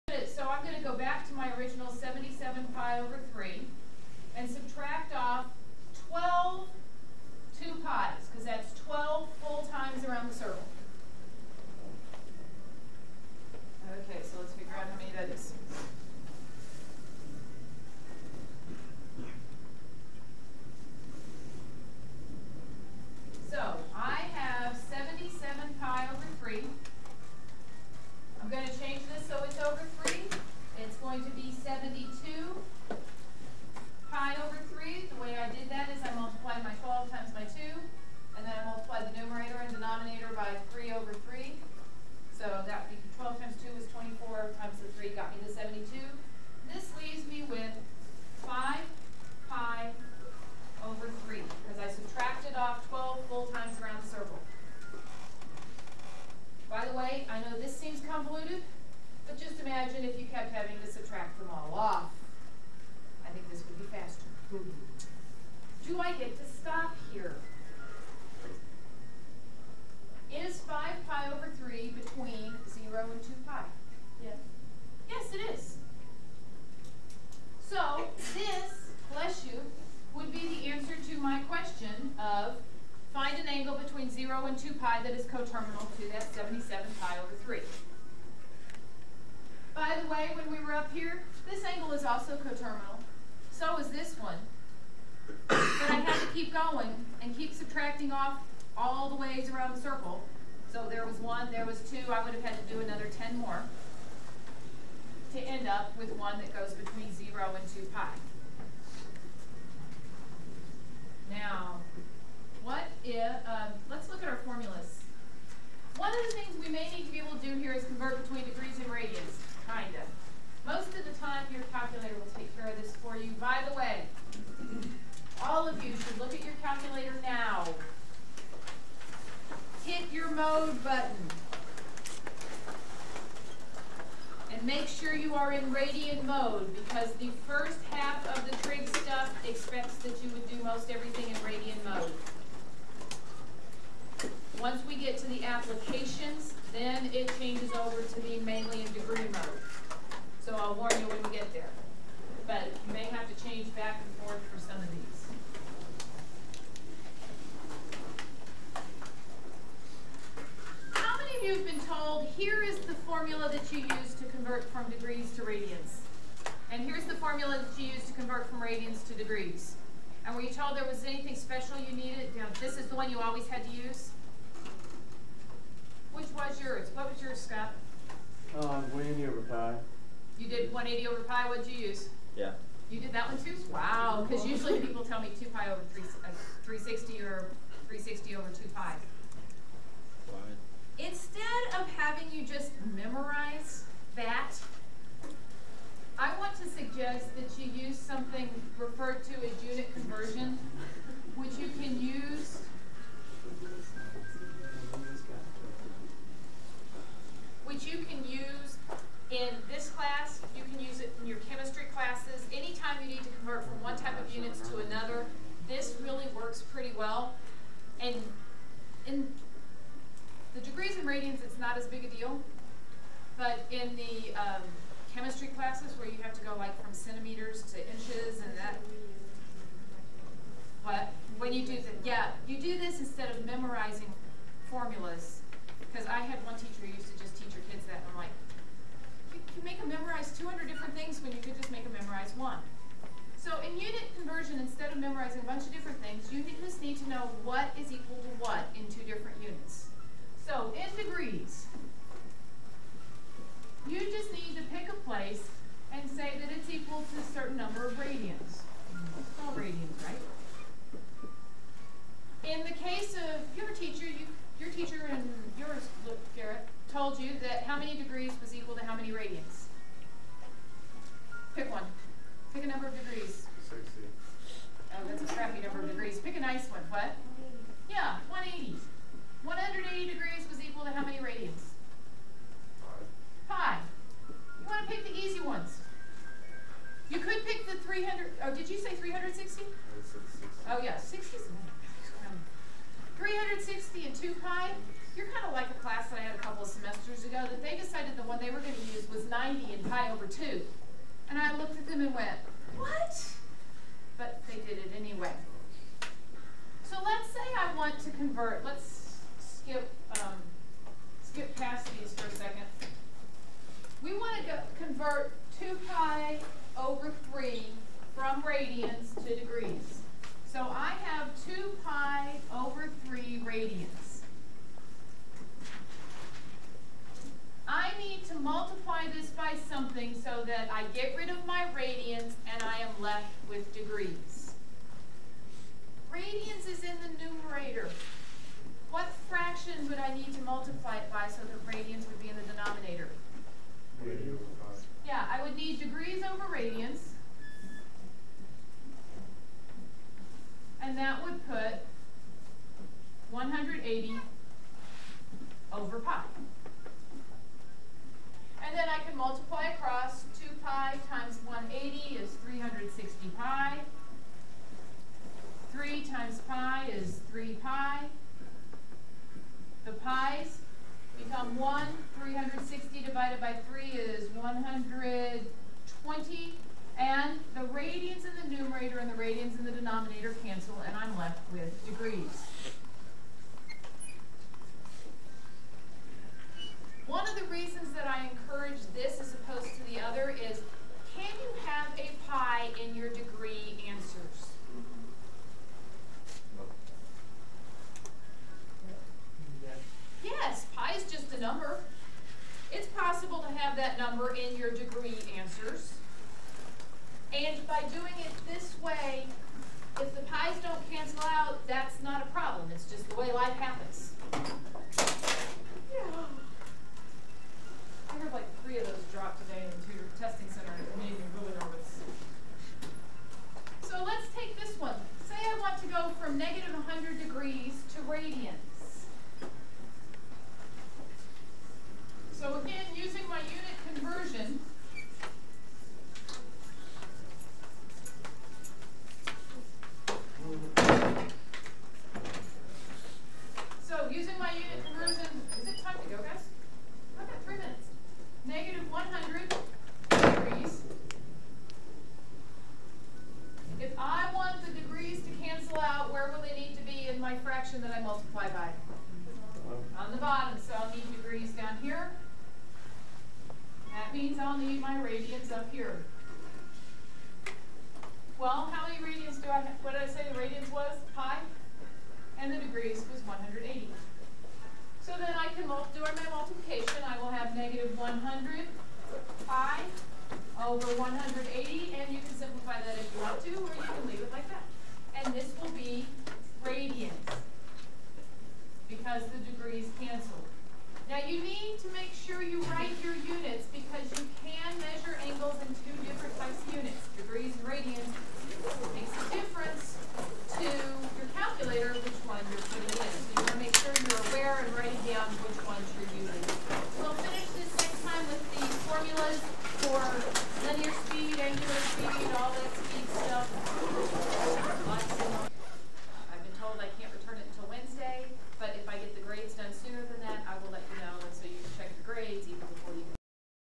Angles and Radian Measure (part 3)
This is the beginning lecture on the unit circle, angles, and measuring angles.